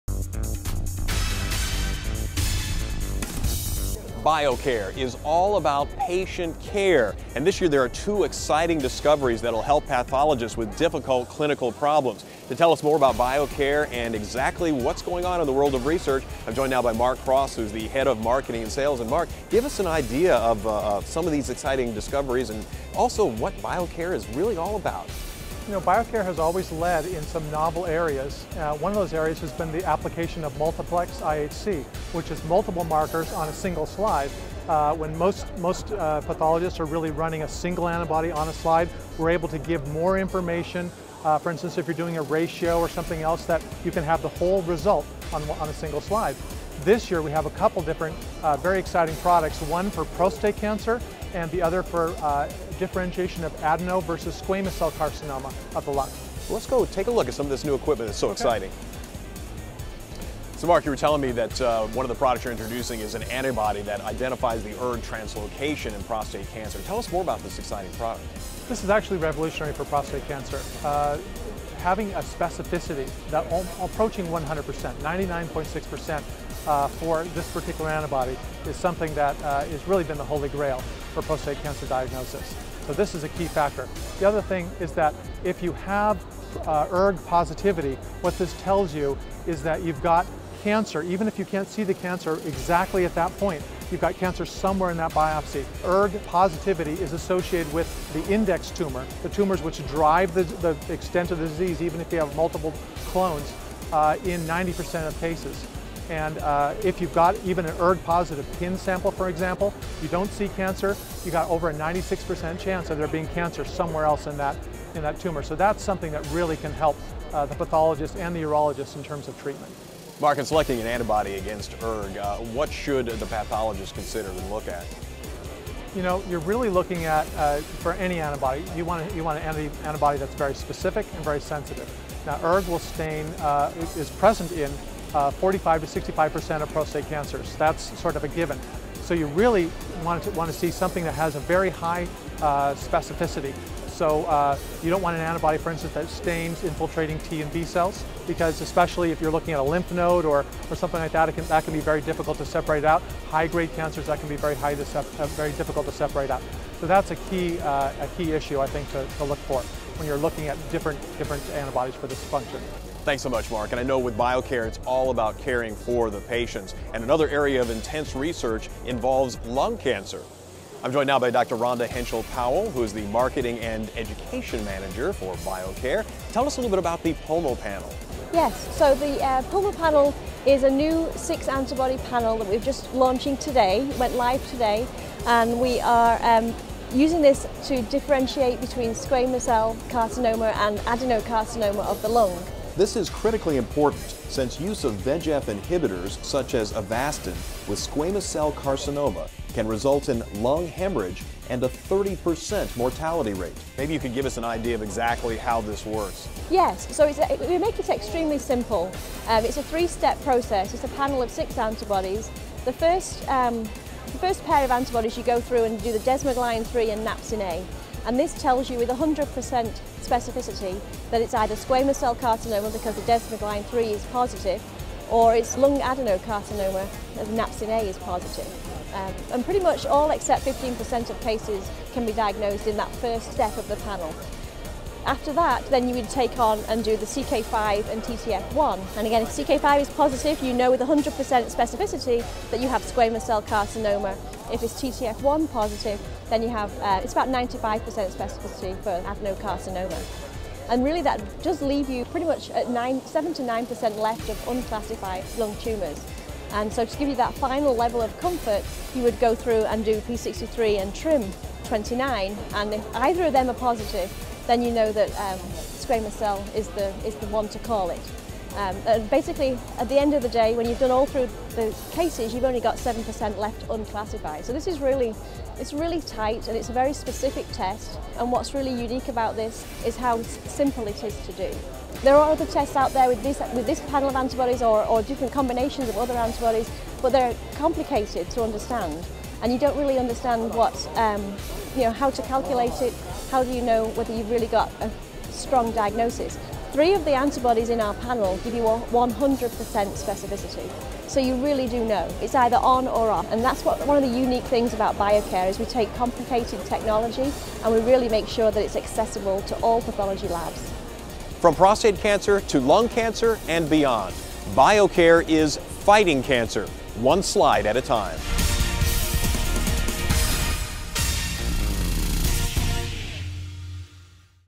Biocare Medical USCAP 2011
See the novel products Biocare is introducing at USCAP 2011 such as ERG for prostate cancer and the PulmoPanel&trade; for lung adenocarcinoma and squamous cell carcinoma differentiation.